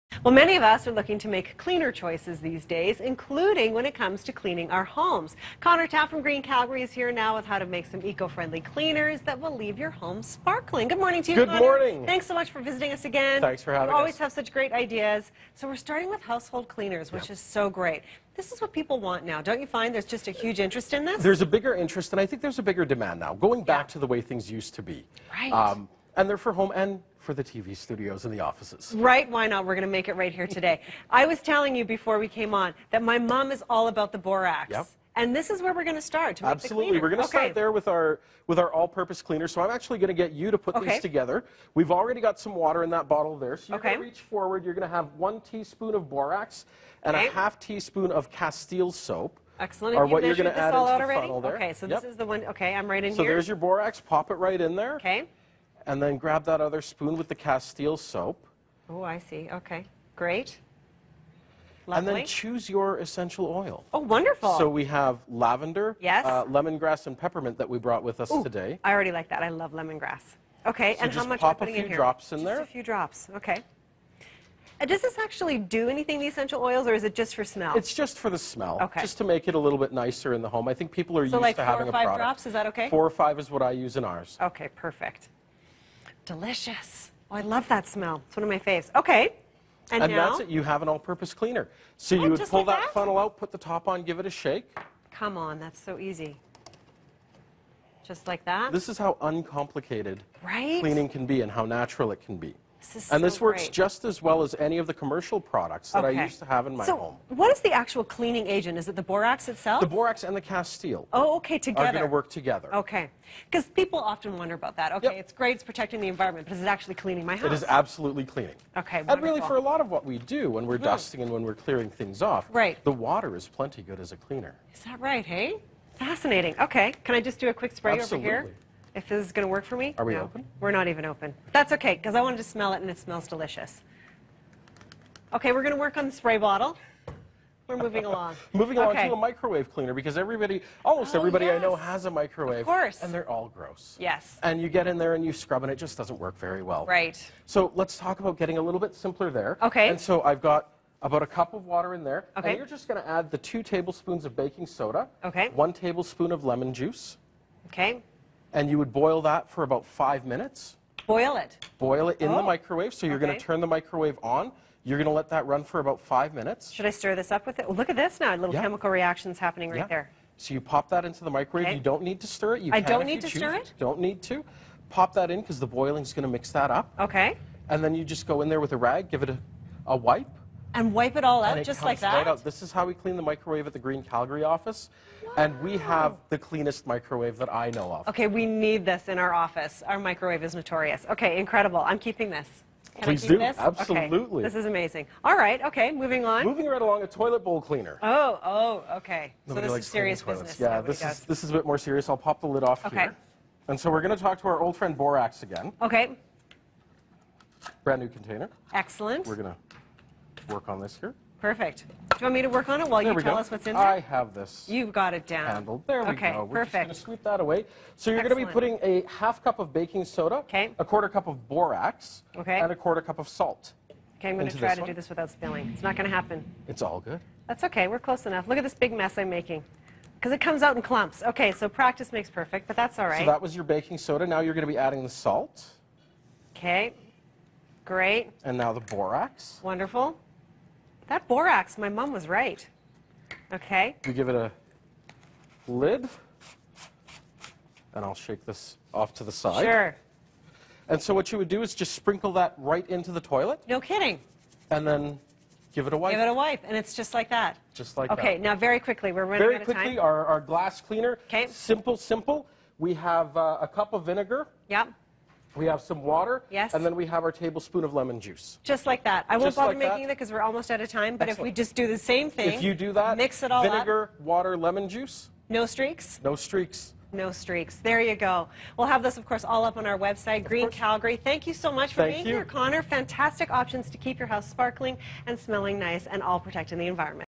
Going Green with Your Cleaning Products
Conor Tapp from Green Calgary shows us what products we should be using for cleaning if we want to be easy on the environment and ourselves.